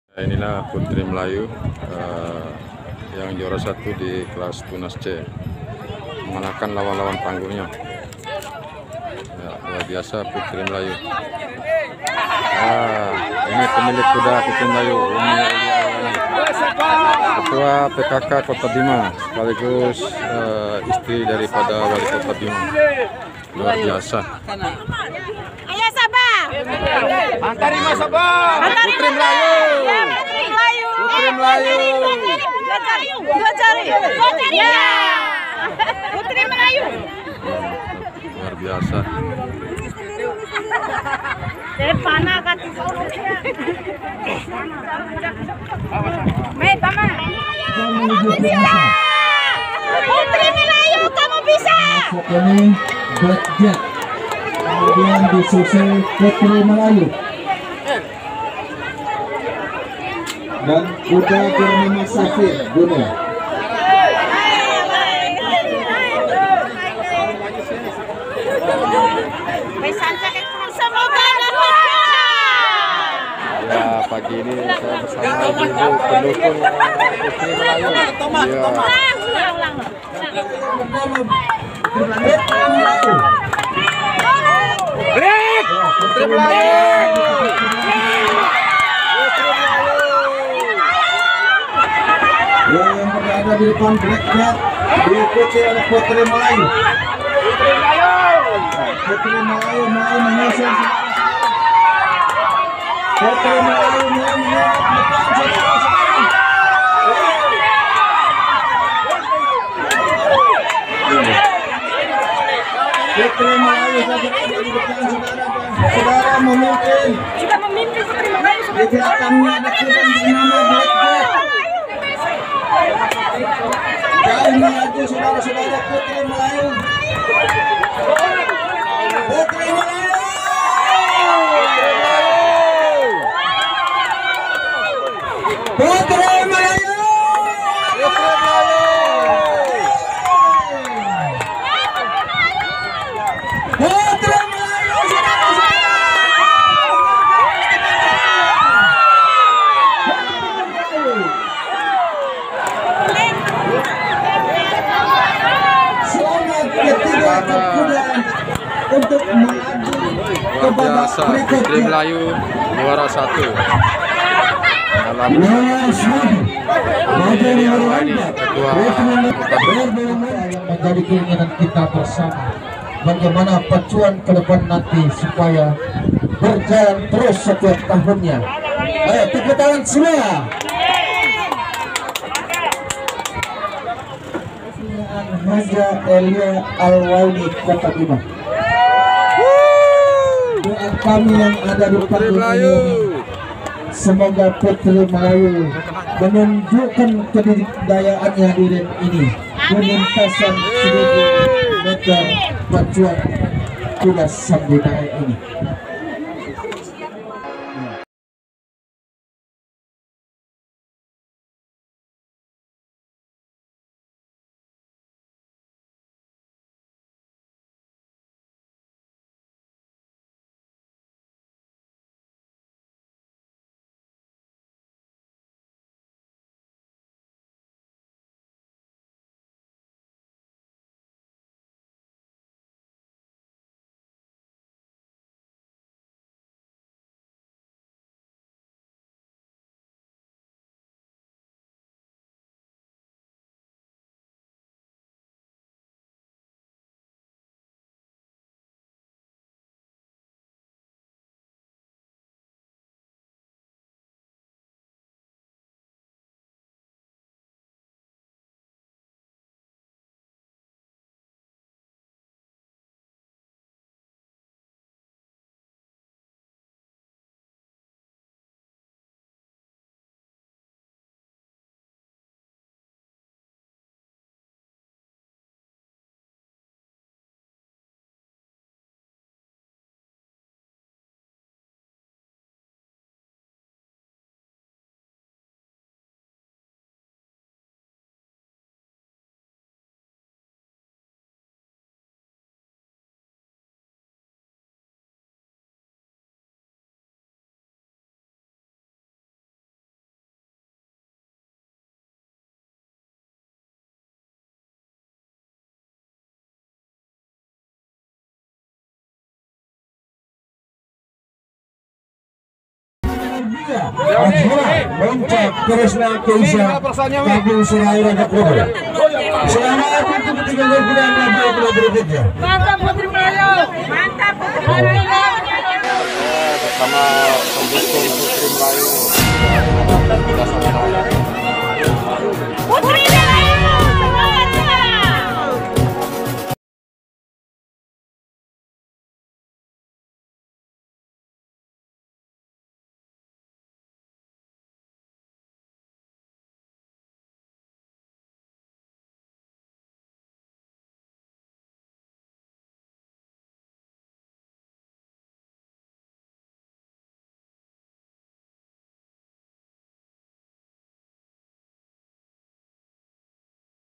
Penonton Histeris Akibat Ulah Sang Pesona Arena (Putri Melayu)
Kuda Perempuan Yg Sangat Mempesona Kepunyaan Hj.Elly Alwayni Istri Dari Wali Kota Bima Mengalahkan Semua Lawan Lawan Tangguhnya Di Arena Pacuan Kuda Sambinae Kota Bima,Semua Penonton Terpesona Dengan Kehebatan Sang Juara (Putri Melayu).